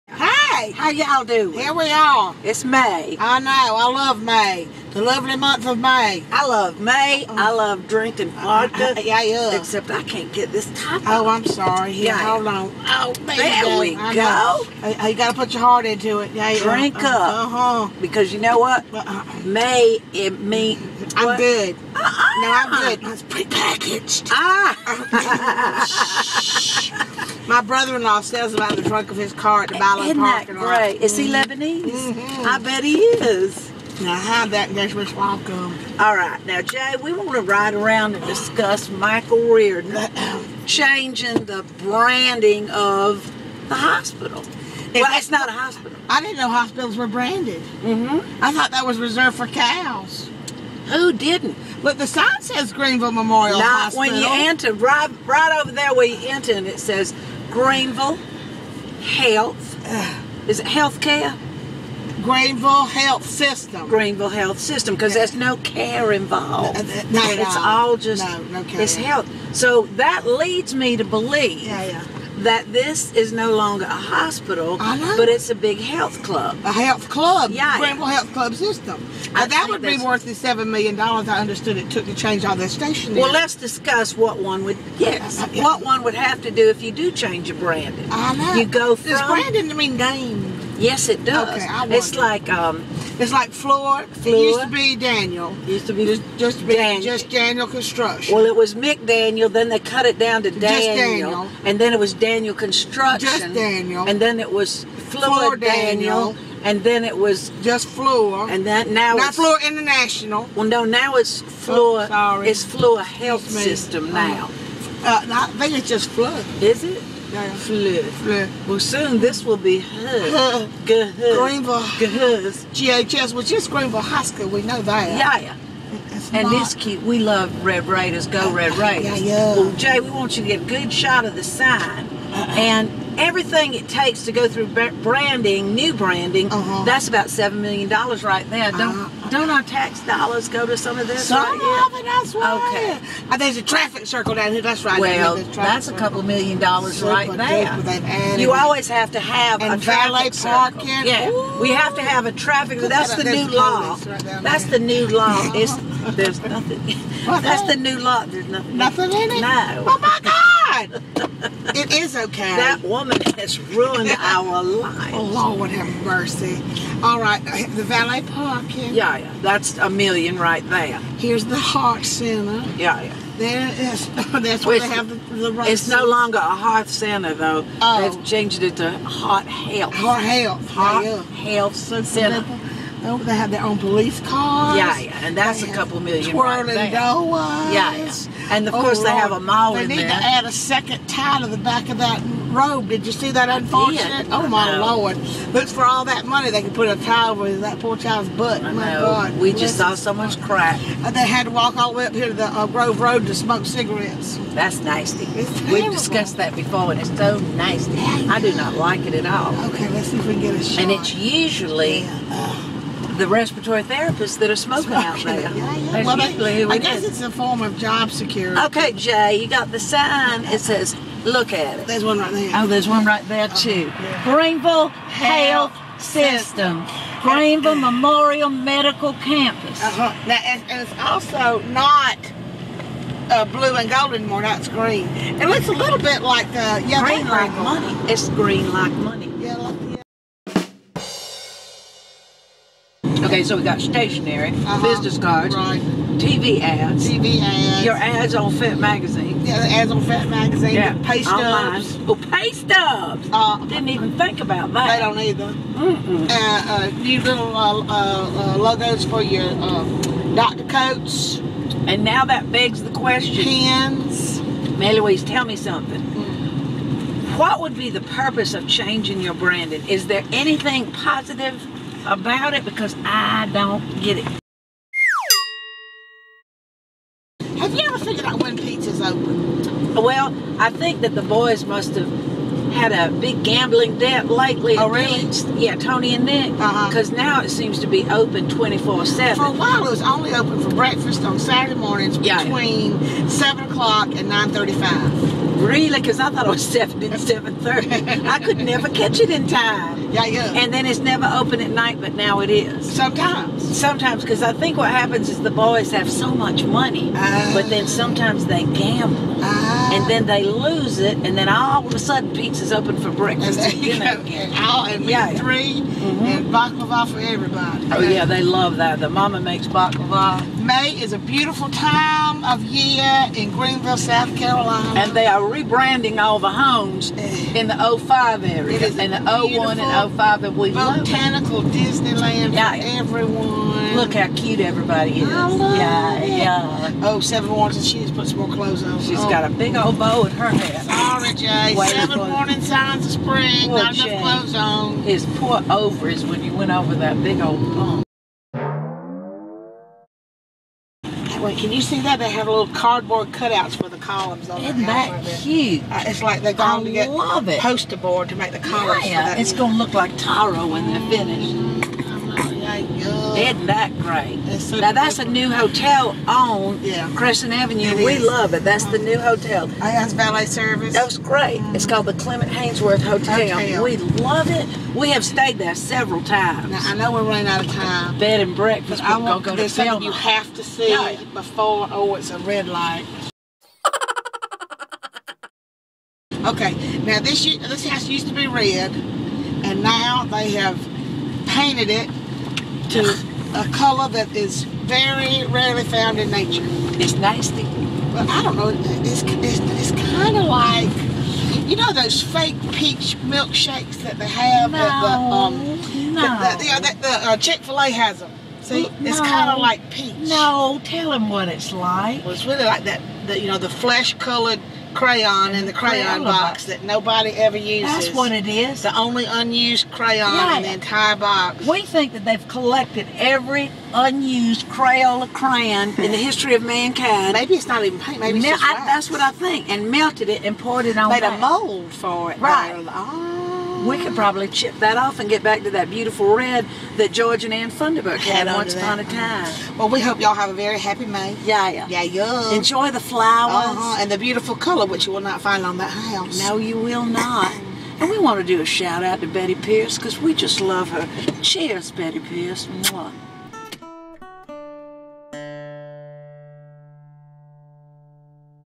The Cafe & Then Some Ladies - May 2013
Rebranding, Pete's and a bad choice on house paint.  The CATS ladies give us a healthy dose of humor this month.